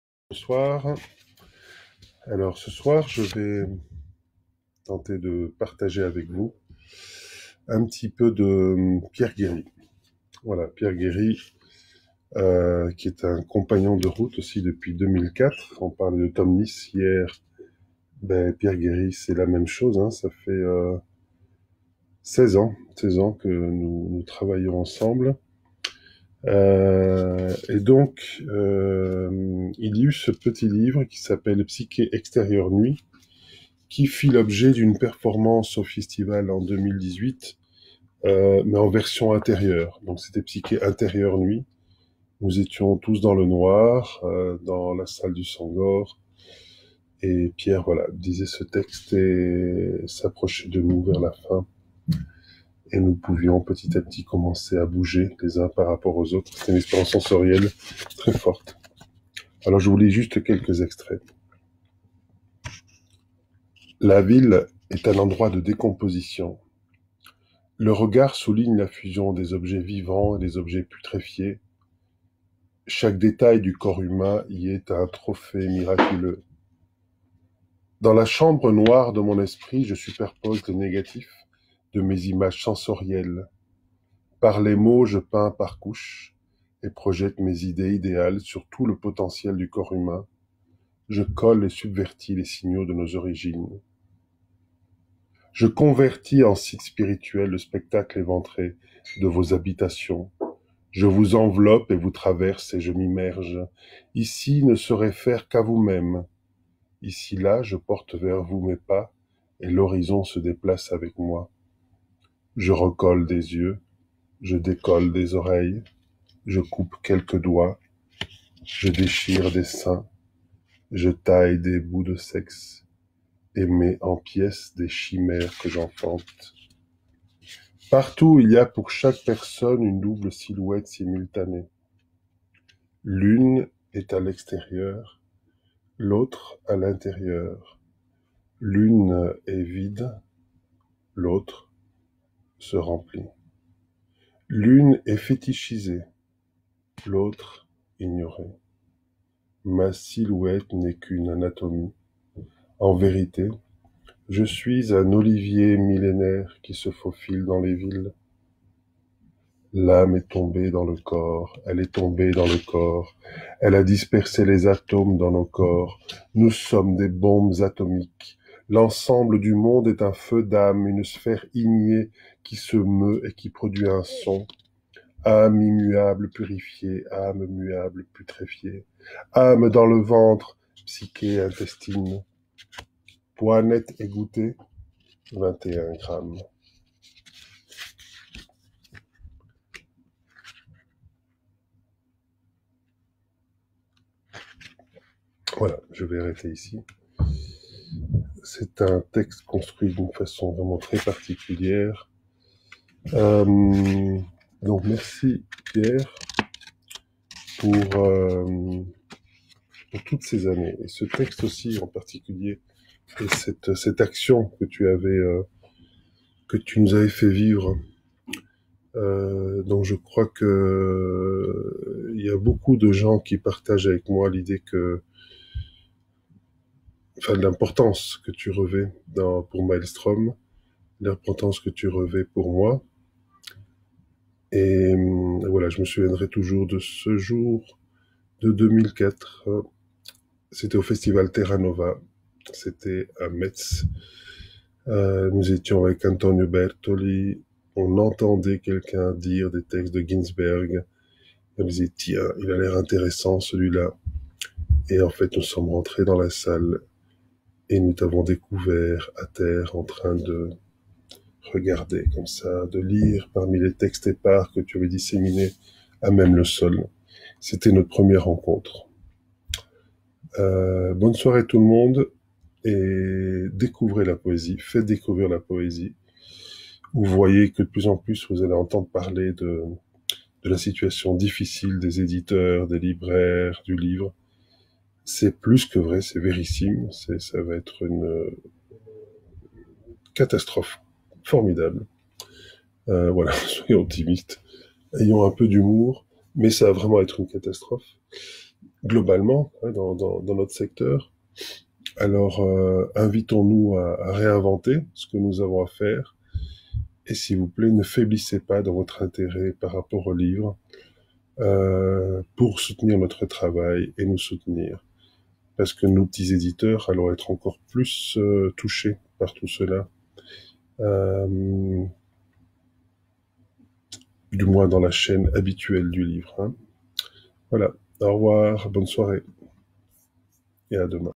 Poésie d’au-delà du confinement #36 – « Psyché Extérieur Nuit » de Pierre Guéry
Poésie d’au-delà du confinement #36 – Un extrait de « Psyché Extérieur Nuit » de Pierre Guéry (éd. maelstrÖm reEvolution).

Lecture par David Giannoni
Retrouvez toutes les vidéos de la série « Poésie d'au-delà du confinement » ici sur notre chaîne Youtube